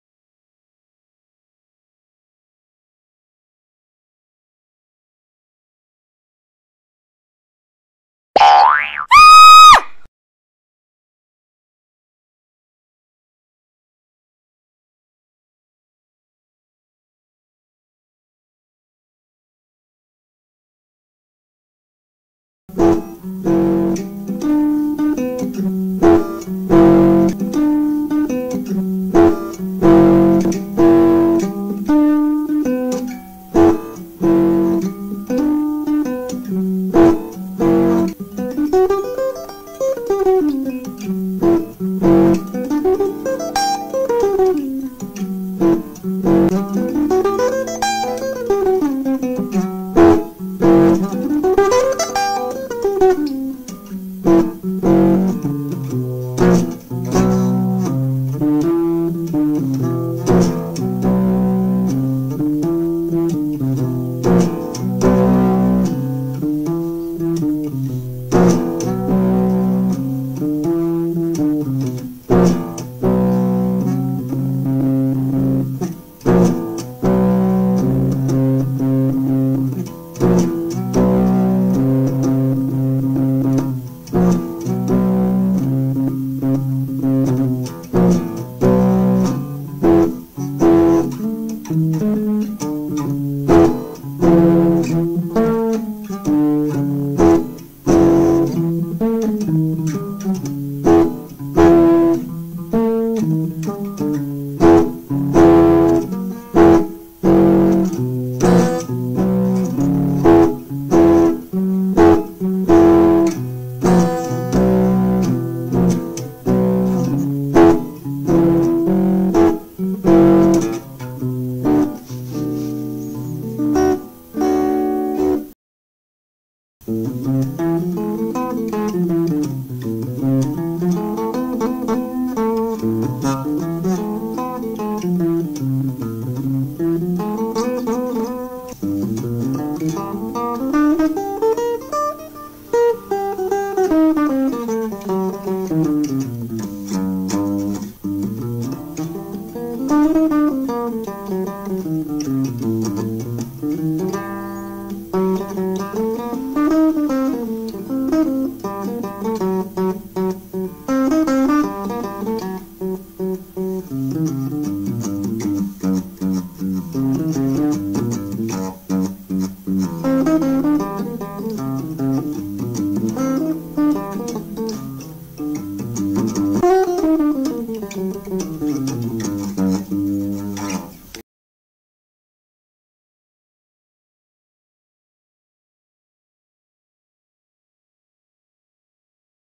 ORANGE CRUSH 20 | Overdrive | Dirty Channel
Dirty Channel, a slight Overdrive, no Reverb
Neck Humbucker, Jazz-Rock Tone